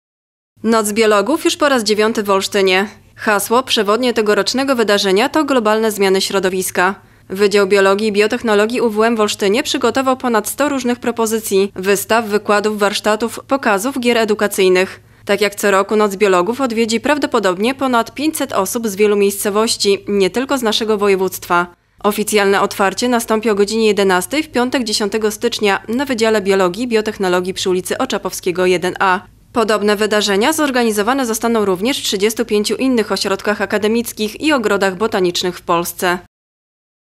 Noc Biologów już po raz dziewiąty w Olsztynie
Hasło przewodnie tegorocznego wydarzenia to  „Globalne zmiany środowiska”.